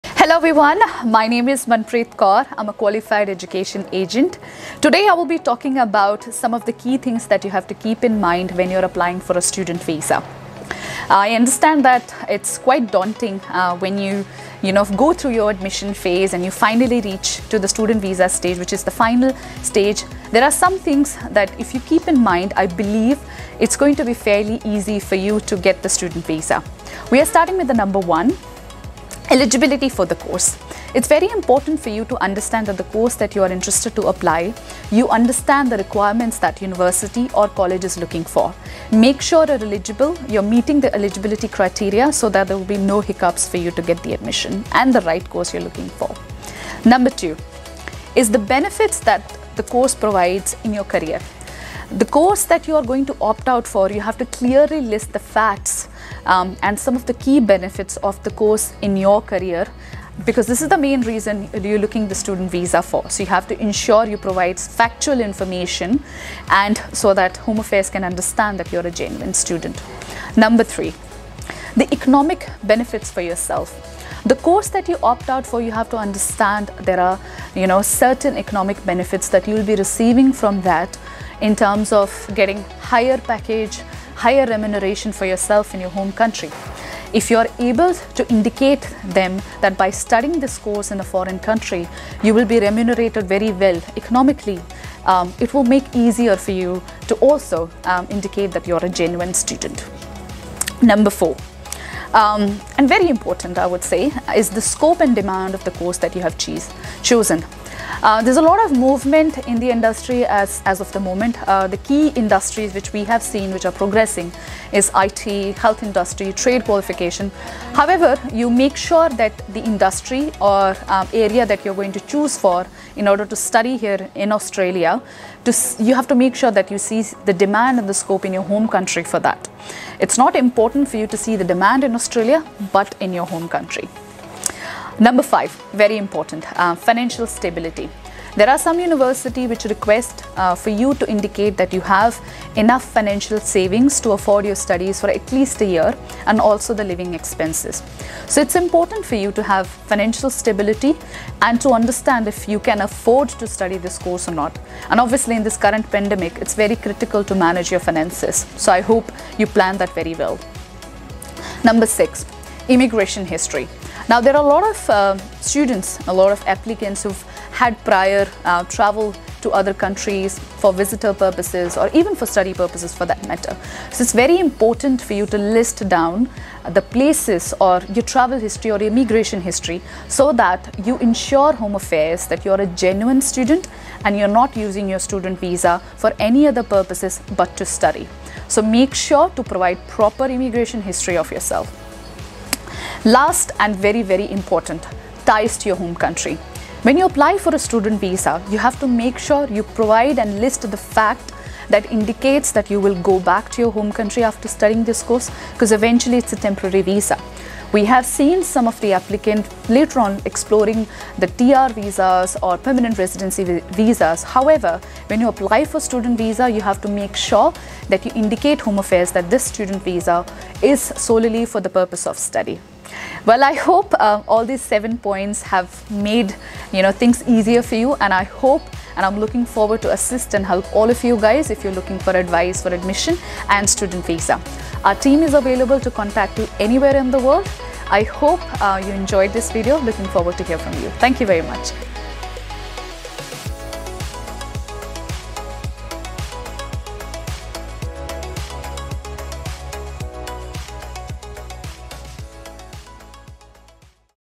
7 key student visa requirements for Australia
It is a daunting task if you fail to understand the criteria of a student visa to Australia.

Do not miss out on 7 key requirements before applying for the visa.

Qualified Education Agent Manpreet Kaur lists them out—
✔ Eligibility criteria
✔ Benefits of the course for a career
✔ Economic benefits the course brings you
✔ Demand of chosen course in the home country
✔ Financial stability
✔ Immigration history
✔ Ties to home country

Drop your queries in the section below and we will get back to you.